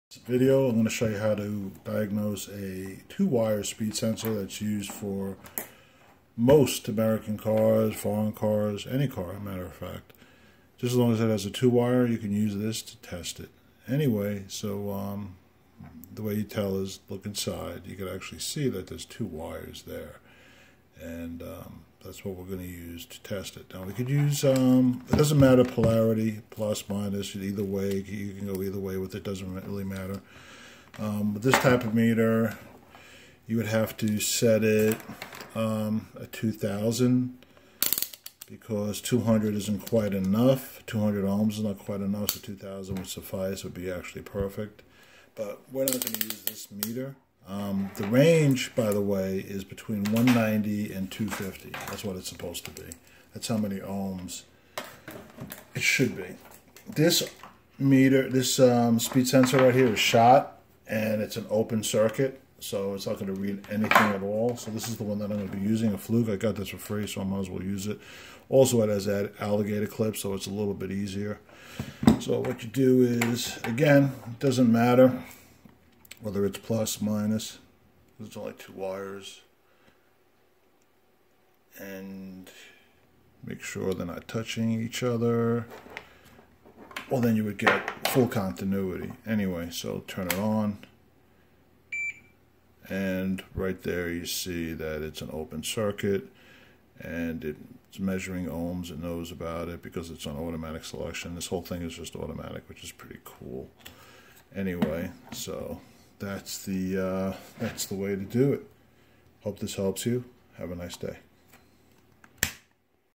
Testing For All 2 wire Wheel Vehicle Speed Sensors ABS VSC Etc
Using a Digital Volt-Ohmmeter (DVOM), measure the resistance (ohmmeter function) between the sensor terminals. If the resistance is 190–250 ohms, the sensor is okay.